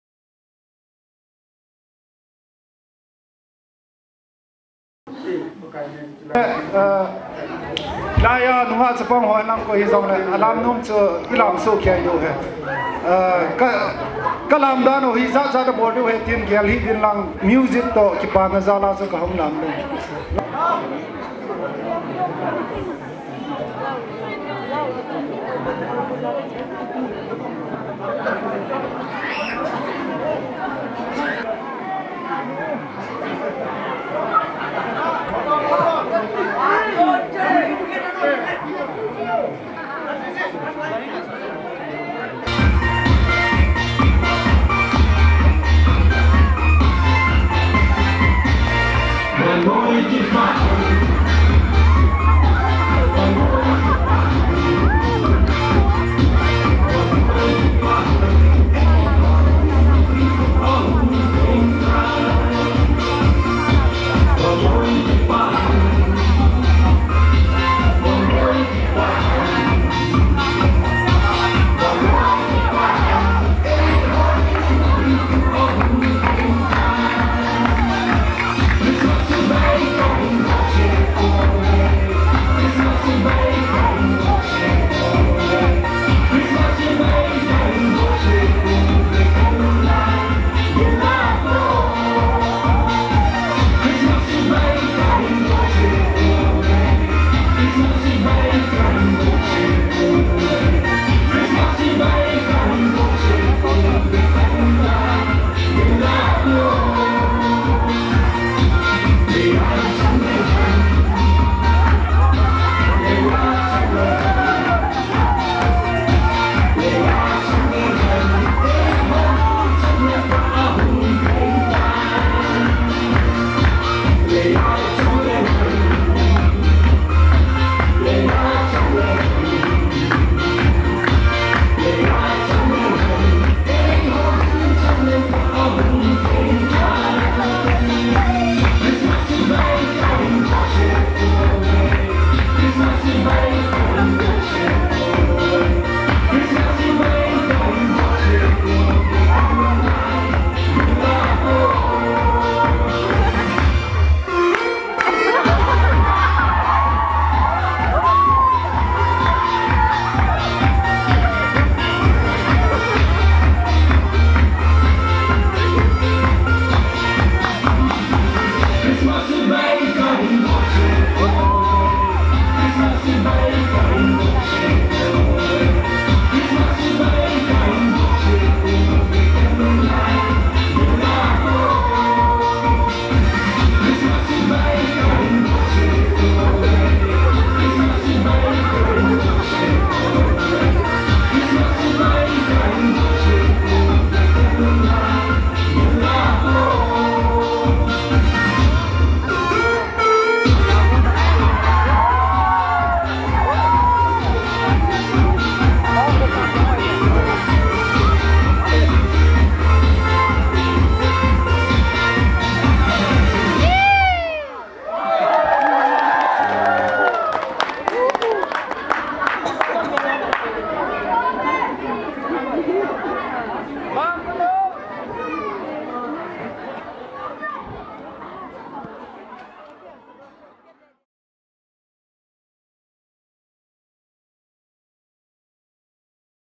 Thongkhosiem @Semboi Haokip & Party perfomed an Awsome Dance in Christmas 2017
ISL INDIAN PLAYER Mr.Thongkhosem @Semboi Haokip performed a beautiful Awsome Dance in his home town Gangpijang Village during celebration of Christmas 2017.